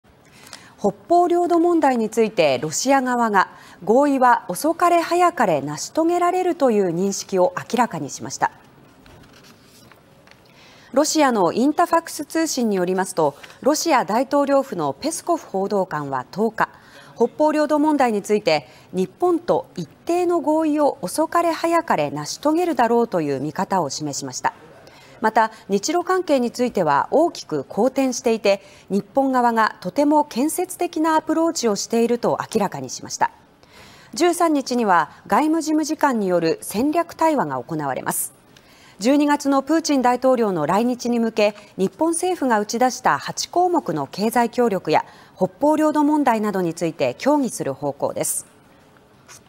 「北方領土、遅かれ早かれ合意」ロシア大統領報道官(16/10/11)
北方領土問題について、ロシア側が「合意は遅かれ早かれ成し遂げられる」という認識を明らかにしました。　ロシアのインタファクス通信によりますと、ロシア大統領府のペスコフ報道官は10日、北方領土問題について、日本と「一定の合意を遅かれ早かれ成し遂げるだろう」という見方を示しました。